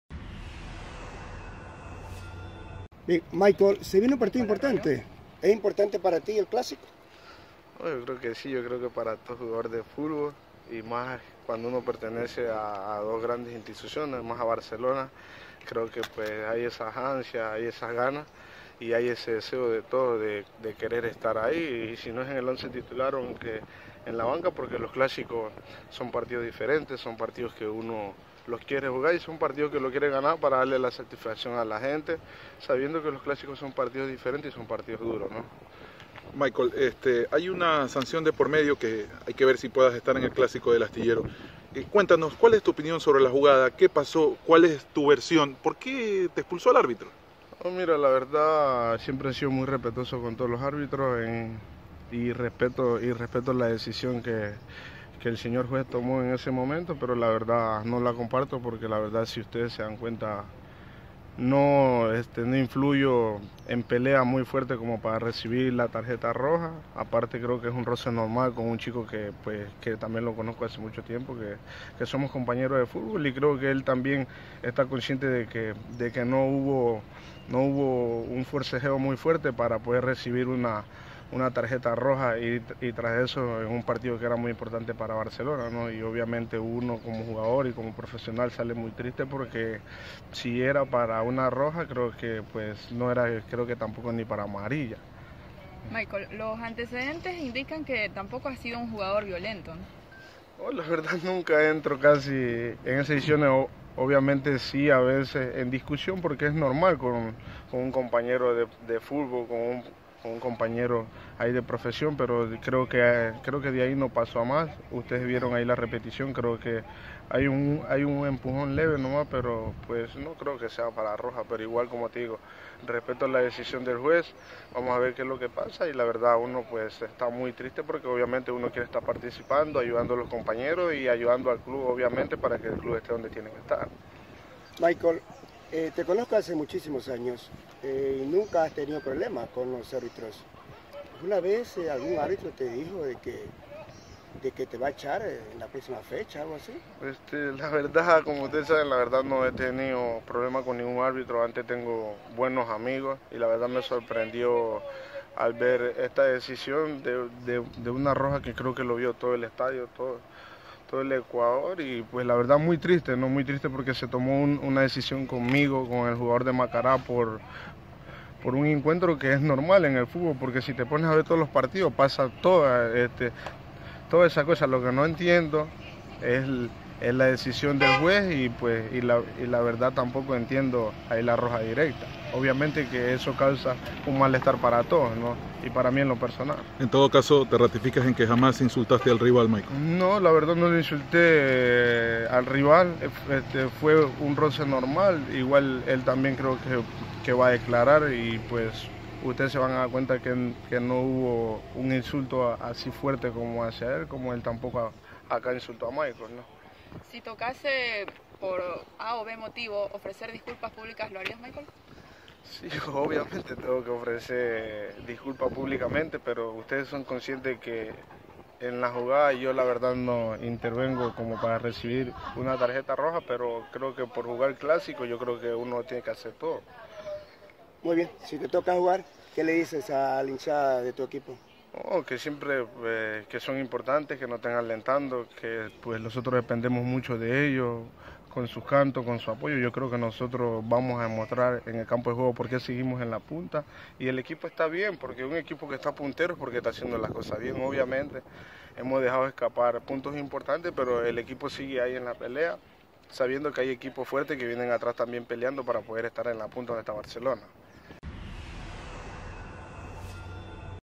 Declaraciones de Michael Arroyo: "Quiero jugar el Clásico ante Emelec"
Cortesía Cristhian Carrasco